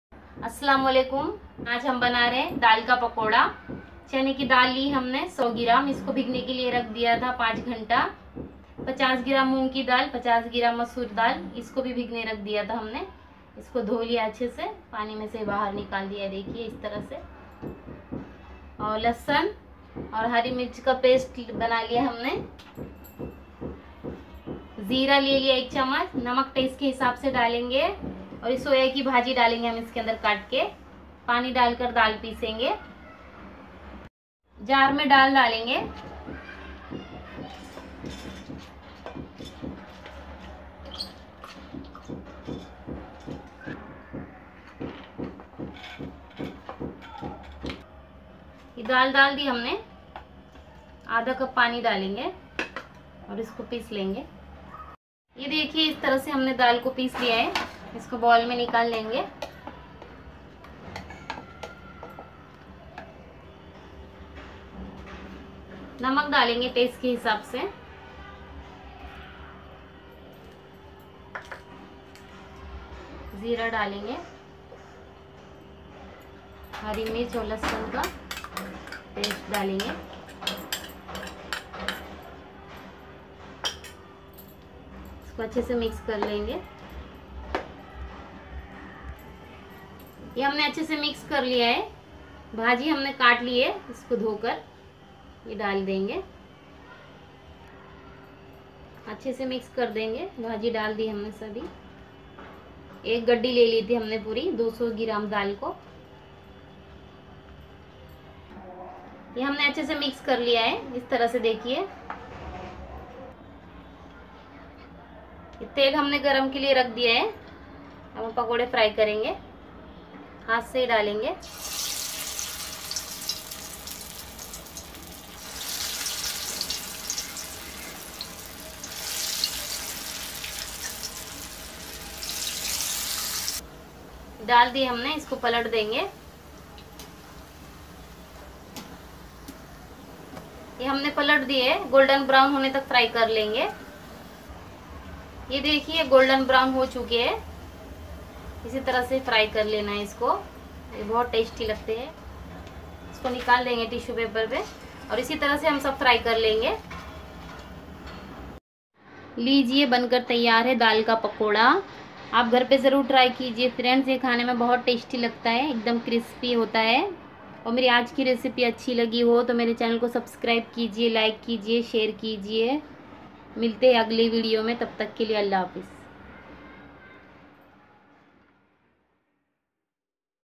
| बारिश के मौसम में बनाए हरियाली पकोड़ा कुरकुरा और टेस्टी भजिया बनाने का परफेक्ट नया तरीका |
| बारिश के मौसम में बनाए हरियाली पकोड़ा कुरकुरा और टेस्टी भजिया बनाने का परफेक्ट नया तरीका | Soya Ki Bhaji Ka Pakoda